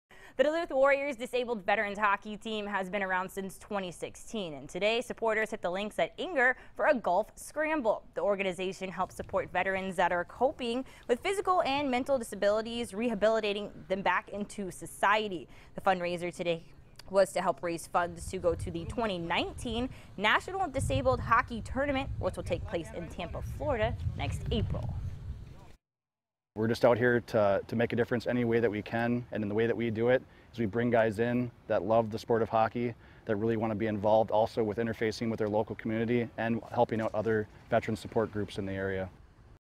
Duluth Warriors Golf Scramble
Duluth Warriors Disabled Veterans Hockey Team Golf Scramble
29 June 2018 @ Enger Park Golf Course